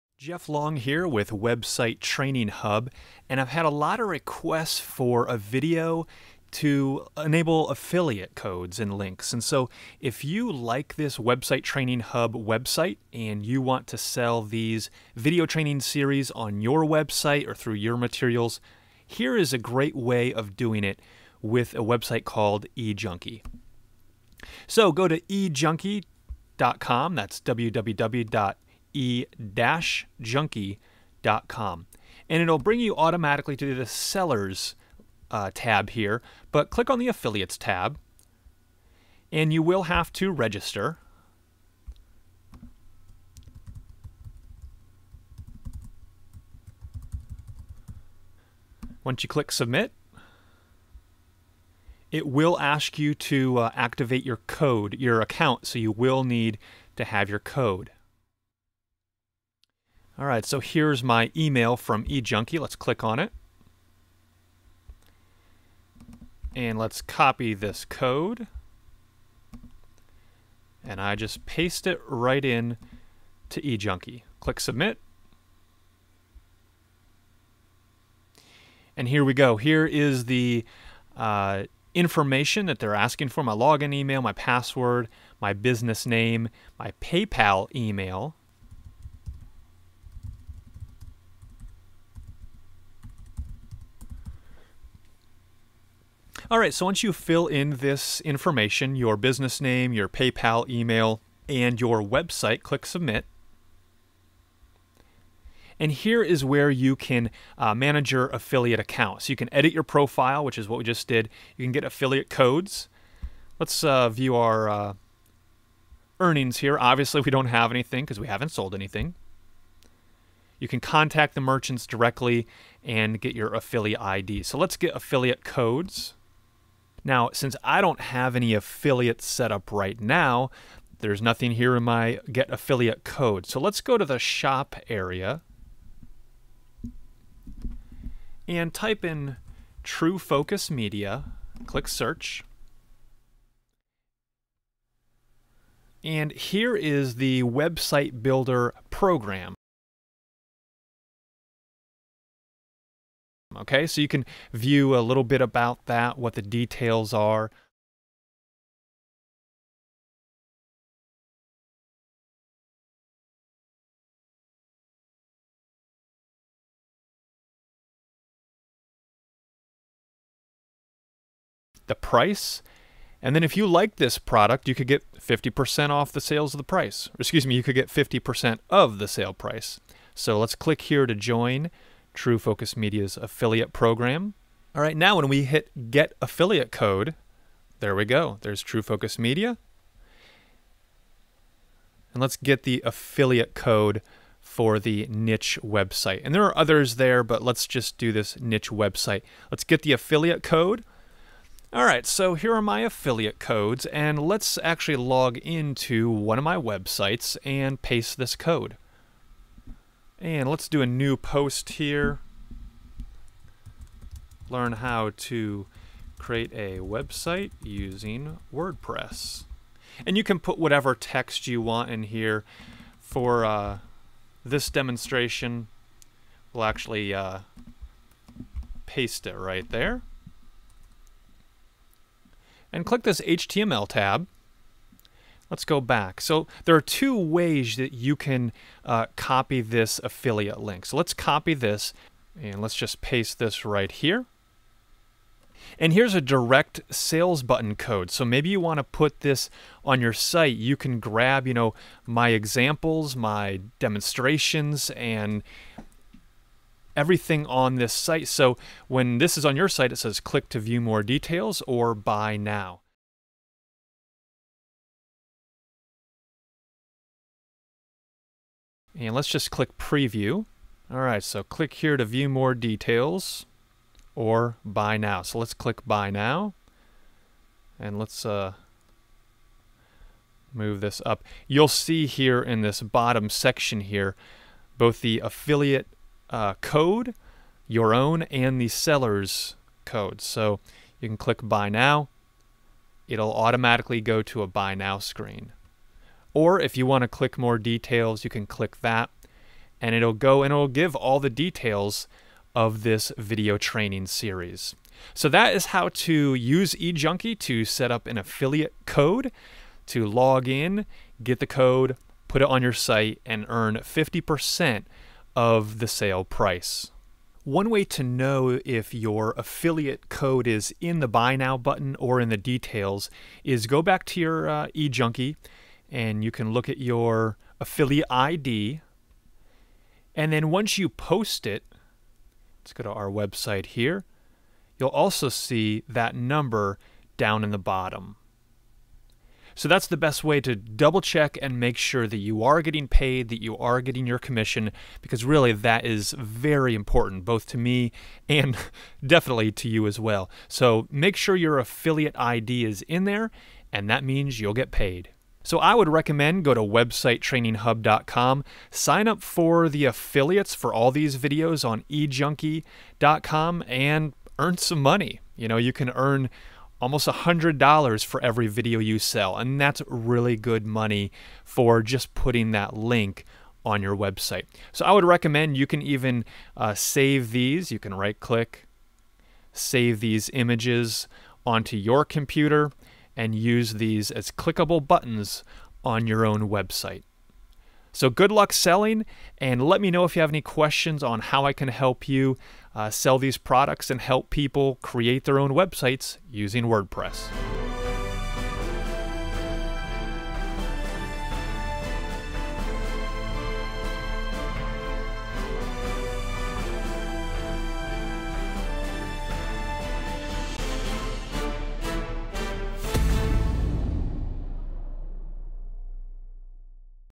How to sign up for an affiliate account with e-junkie.com
shows you how to sign up to sell Wordpress training videos by using

Get training videos about how to set up your own Wordpress website and blog